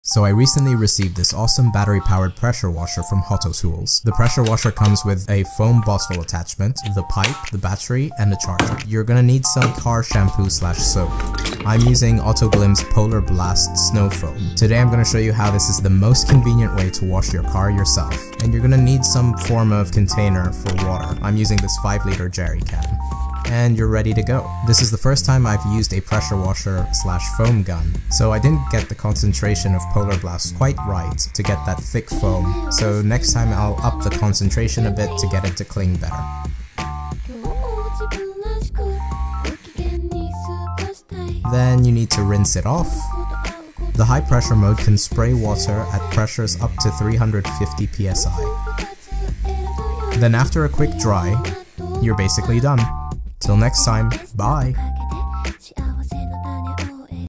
Using a FOAM-CANON for the first time EVER!
Using the HotoTools pressure washer for the first time ever!
I’ve never used a pressure washer to clean my car before, as I typically take it to the car wash, but after Hoto reached out to me to try their pressure washer, I decided to give it a go.
It is actually a lot more powerful than I had expected with pressures up to 350psi!
As I don’t have an available water supply outdoors, it is great to use be able to use a jerrycan/bucket of water to clean the car!
It also has this great foam gun attachment allowing you to add some car shampoo in there.
And the nozzle of the pressure washer is awesome, allowing you to change mode on the go!